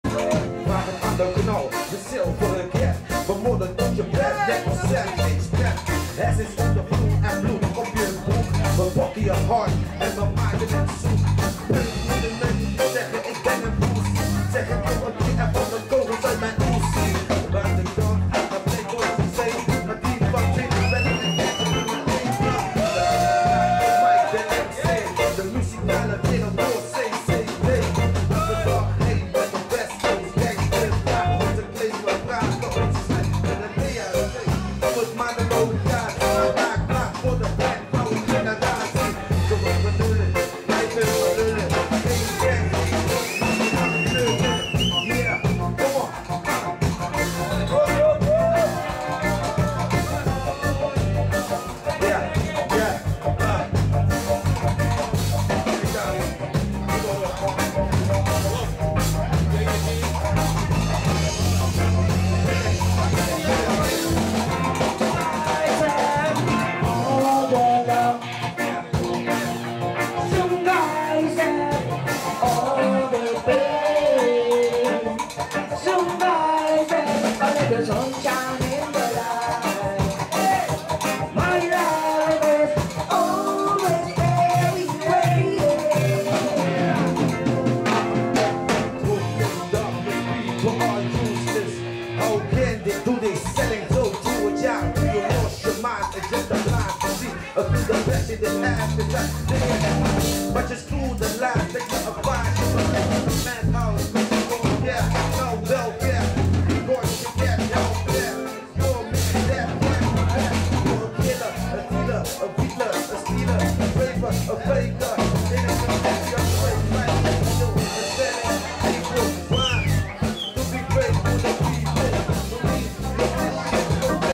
Reggae band Delft @ De Pater The Hague
Open mic met Reggae band Delft @ café de Pater The Hague.